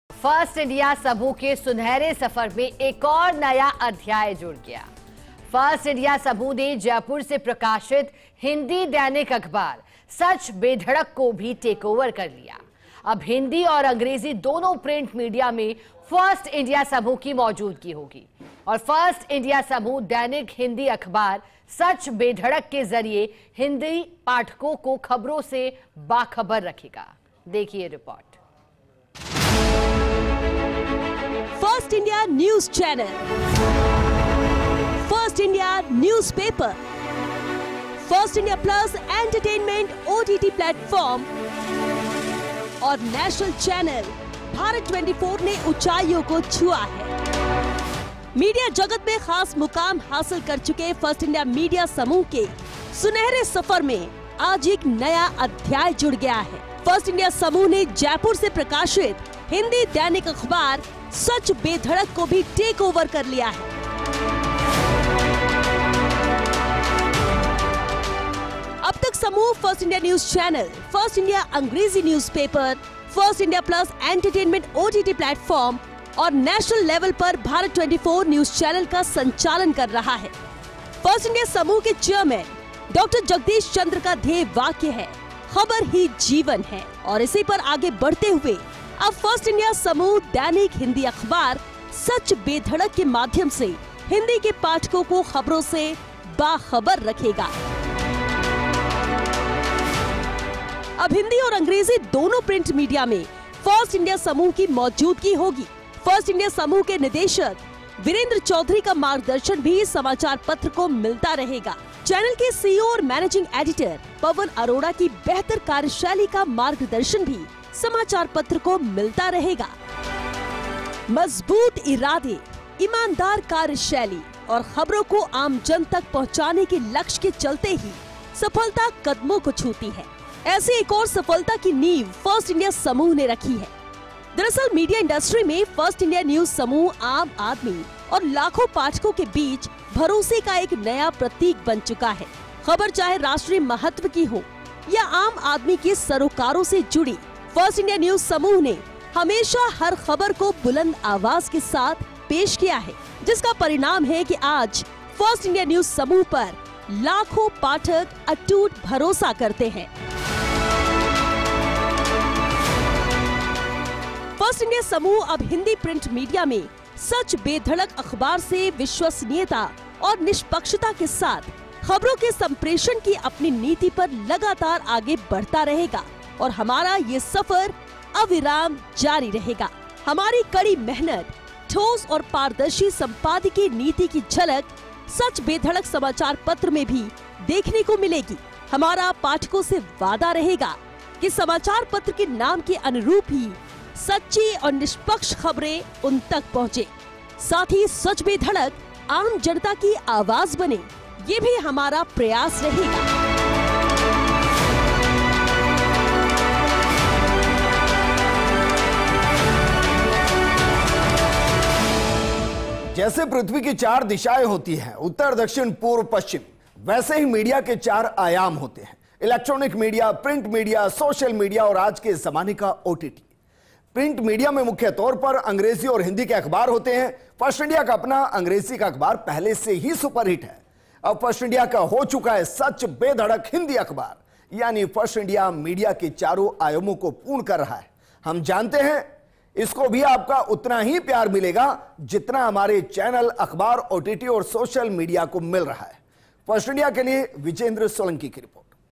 First India मीडिया समूह के सुनहरे सफर में जुड़ा एक नया अध्याय, अखबार 'सच बेधड़क' को किया टेकओवर
About this Video:

First India मीडिया समूह के सुनहरे सफर में जुड़ा एक नया अध्याय, अखबार 'सच बेधड़क' को किया टेकओवर | 27 September 2024